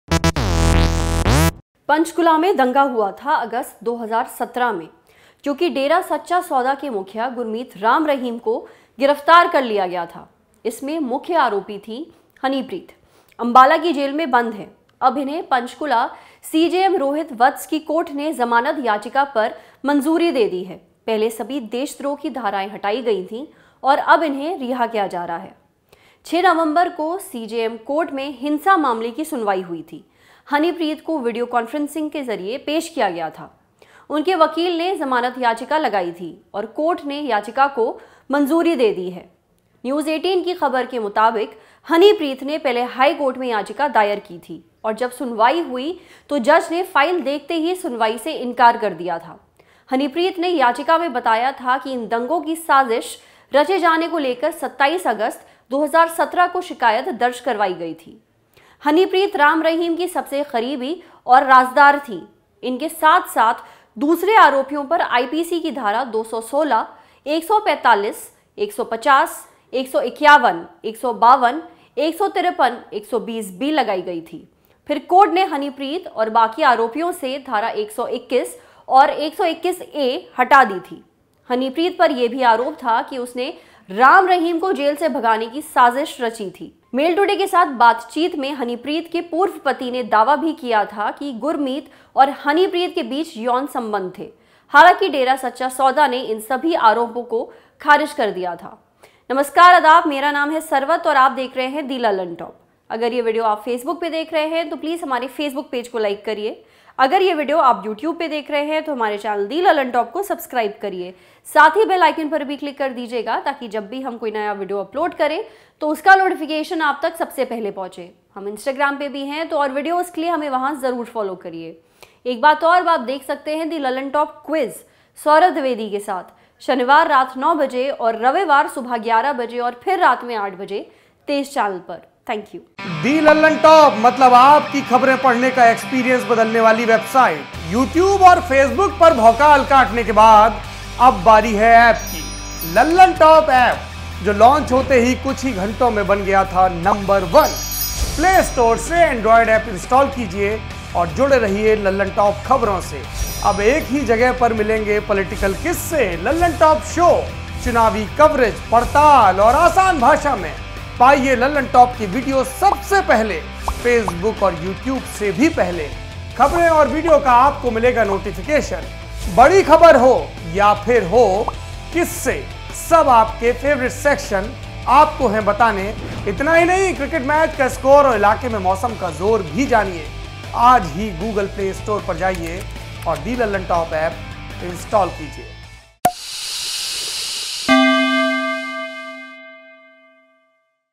Panchkula Riots की मुख्य आरोपी और Gurmeet Ram Rahim Singh की करीबी Honeypreet को मिली Bail
A court on Wednesday granted bail to Honeypreet Insan, adopted daughter of Dera Sacha Sauda chief Gurmeet Ram Rahim, in a case pertaining to violence in Haryana’s Panchkula in 2017. Watch the video for details.